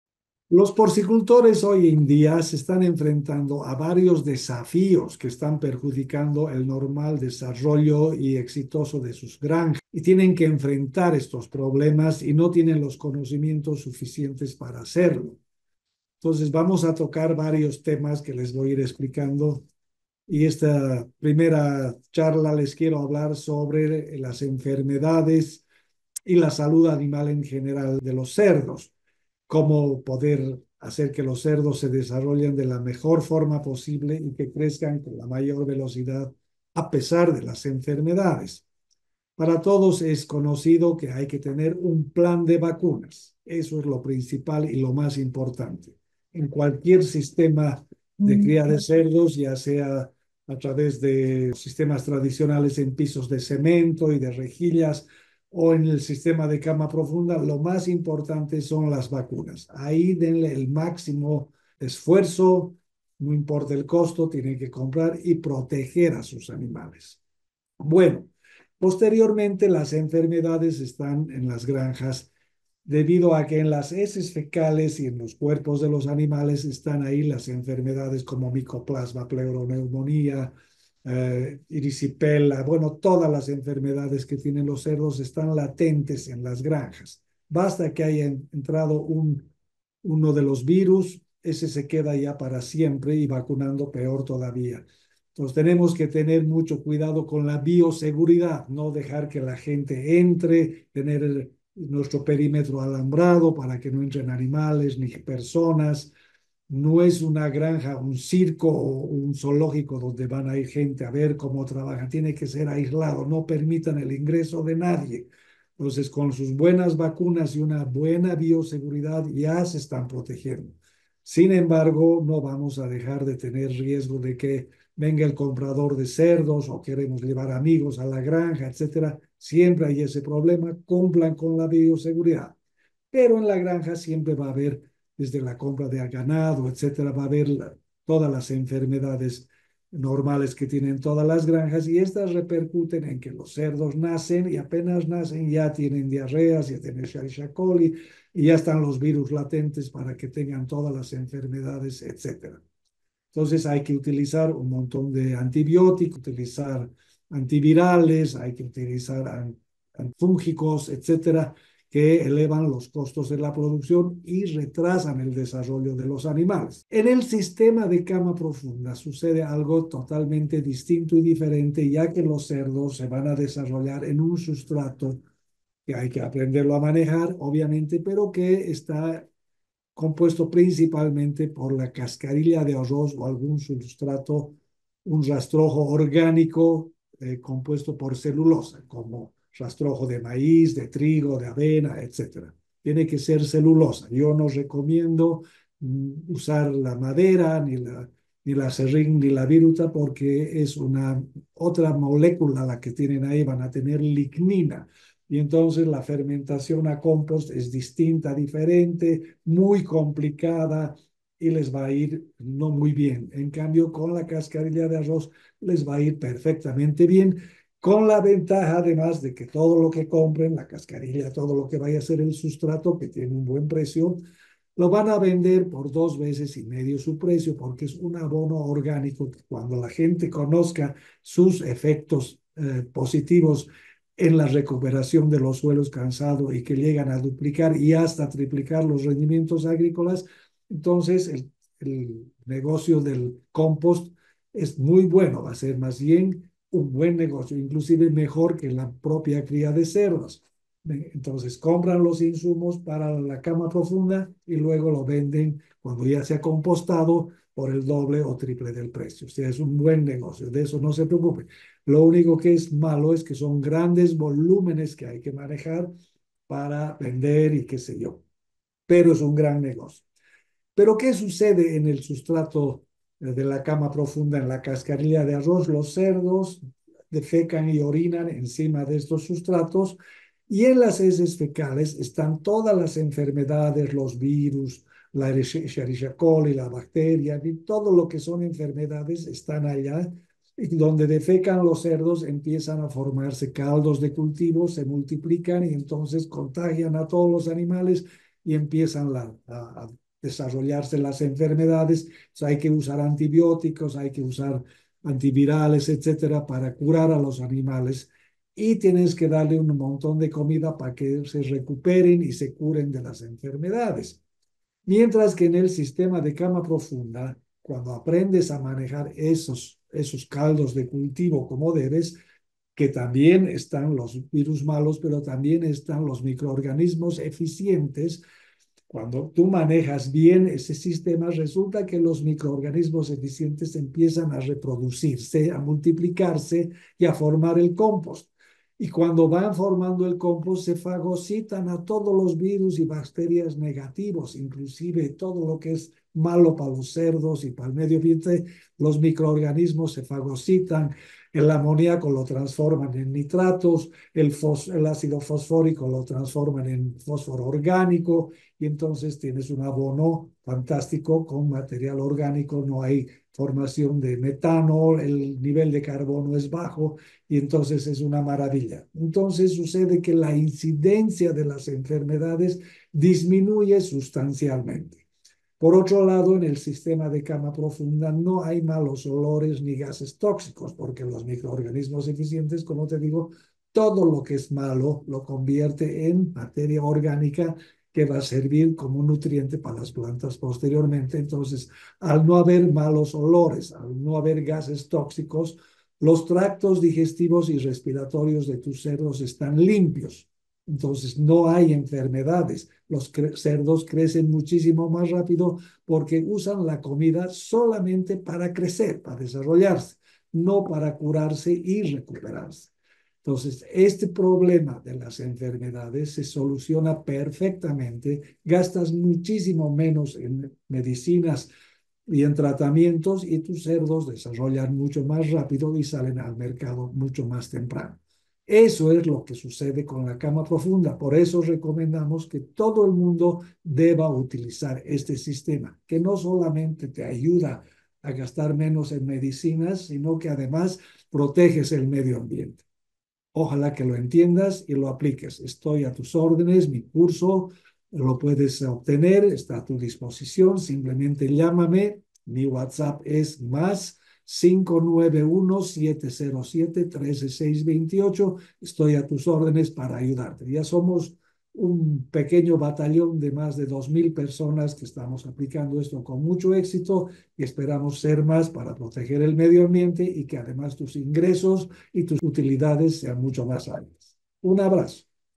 Protegerte de las enfermedades al mínimo costo
Sistema de Cama Profunda por Gonzalo Zalles te ayudará a disminuir sustancialmente la incidencia de las enfermedades al mínimo costo